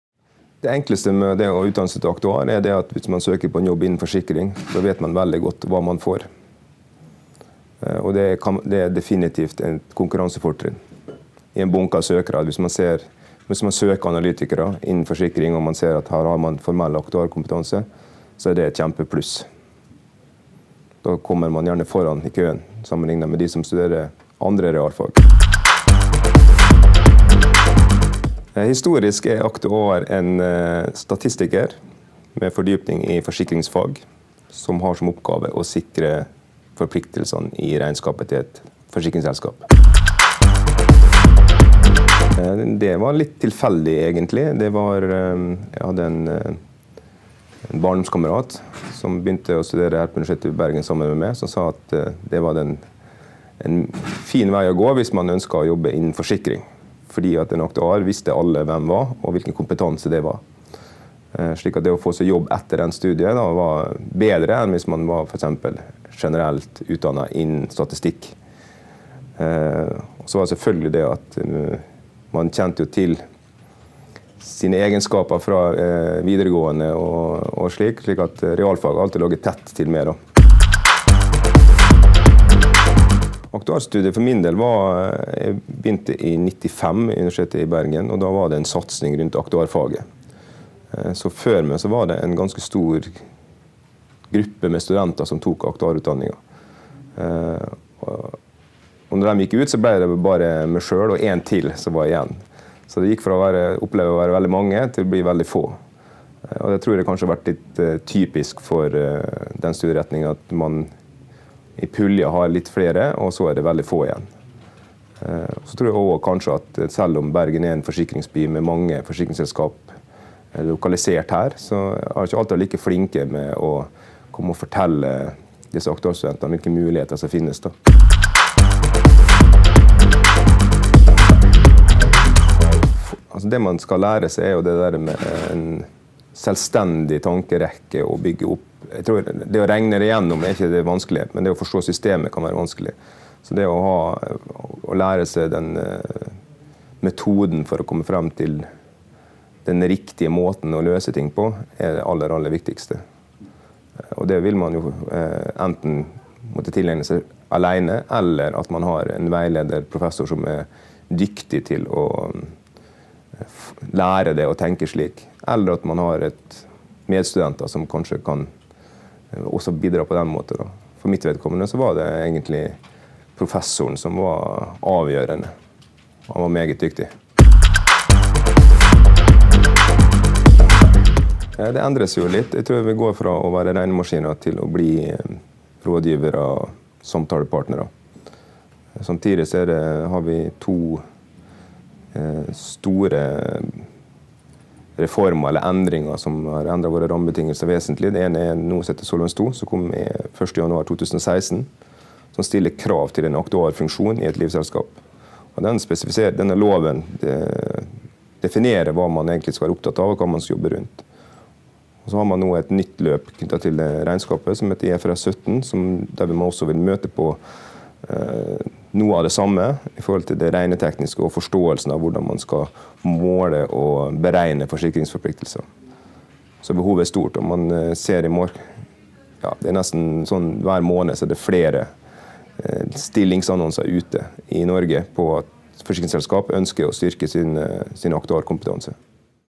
Finansbransjen etterspør aktuarer
Korleis skal oljefondet investere for å få høgast mogleg avkastning, men samtidig ha lav risiko for tap? Kva bør bilforsikringa di koste? Aktuarfaget bruker statistiske og matematiske metodar for å løyse slike problemstillingar innan forsikring og finans.